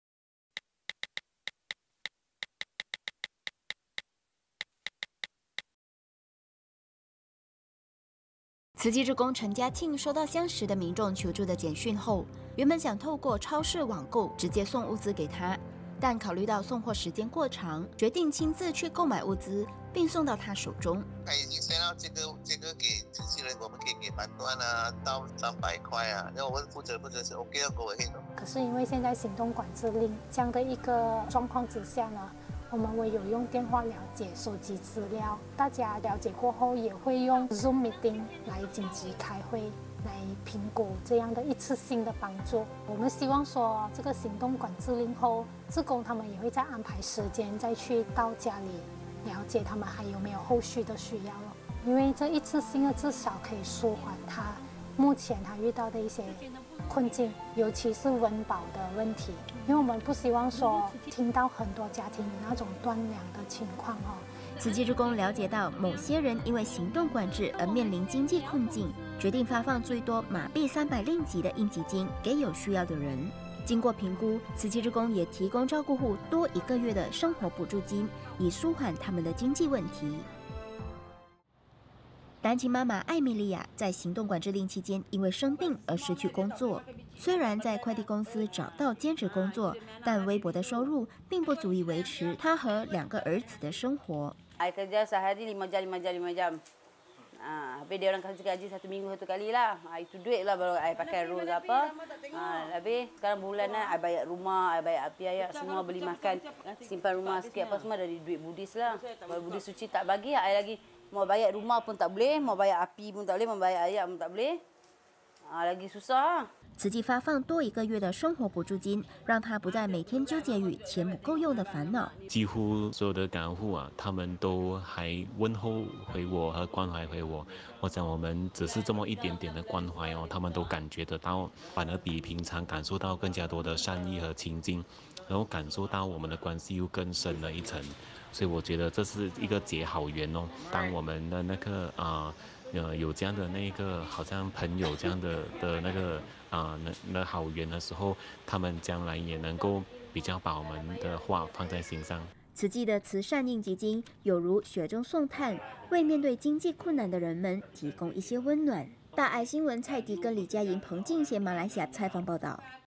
20200418【慈善】這疫關頓失收入 生活補助金解困
馬來西亞，行動管制令實行後，許多人面臨經濟的困境，照顧戶的經濟情況更因此雪上加霜。慈濟志工瞭解情況後，決定發放最多RM300的應急金給有需要的人。照顧戶方面，慈濟志工也提供他們多一個月的生活補助金，以舒緩他們的經濟問題。

索姆阿姨發送給慈濟志工陳家慶的求助短訊，「早安陳先生，請問慈濟有提供救助嗎？如果有，我需要慈濟的幫助，家裡有8個人，當中有2個老人家。」

慈濟志工陳家慶收到和志工相識的民眾索姆阿姨的求助短訊後，原本想透過超市網購直接送物資給她，但考慮到送貨時間過長，決定親自去購買物資，並送到她的手中。

慈濟志工 陳家慶：「社服同仁已經發給所有慈濟人說明，我們可以提供緊急援助到RM300，我問負責人，負責人說 可以 直接去救人。」

慈濟社服同仁 黃麗環：「但是現在因為行動管制，這樣的狀況之下，我們唯有用電話了解收集資料，大家了解過後，也會用ZOOM來緊急開會，來評估這樣的一次性的幫助，我們希望說 這個行動管制令後，志工他們也會，再安排時間 再去到家裡，了解他們還有沒有後續的需要，因為這一次性的至少可以舒緩，他目前遇到的一些困境，尤其是溫飽的問題，因為我們不希望說，聽到很多家庭有那種斷糧的情況。」

慈濟志工瞭解到某些人因為行動管制而面臨經濟困境，決定發放最多馬幣RM300的應急金給有需要的人。經過評估，慈濟志工也提供照顧戶多一個月的生活補助金，以舒緩他們的經濟問題。

單親媽媽艾米麗亞行動管制令期間因為生病而失去工作。雖然在快遞公司找到兼職工作，但微薄的薪水並不足以維持她和兩個兒子的生活。

慈濟照顧戶 艾米麗亞：「我一天工作五個小時，每個星期拿一次薪水 才有錢可以用，可是現在我都靠慈濟，每個月的生活補助金，來還房租 還水電費，買食物和儲蓄，如果沒有生活補助金的話，我就不能應付這些開銷了，生活也會很困難。」

慈濟發放多一個月的生活補助金，讓她不再每天糾結於錢不夠用的煩惱。

慈濟志工 丘穎曄：「幾乎所有的照顧戶，他們都還問候 回我 關懷回我，我想我們只是這麼一點點的關懷，他們都感覺得到 反而比平常，感受到更多的善意和親近，能感受到我們的關係 又更深了一層，所以我覺得這是一個結好緣，當我們有好像朋友，這樣的好緣的時候，他們將來也能夠，比較把我們的話放在心上。」

慈濟的慈善應急金，有如雪中送炭，為面對經濟困難的人們提供一些溫暖。

蔡迪根 李佳營 彭靜嫺 馬來西亞報導